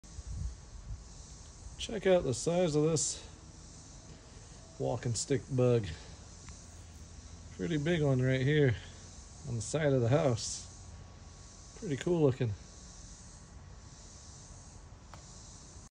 Huge Walking Stick Insect On The Side Of Our House | I Have Not Seen One This Big Before
This was cool to see.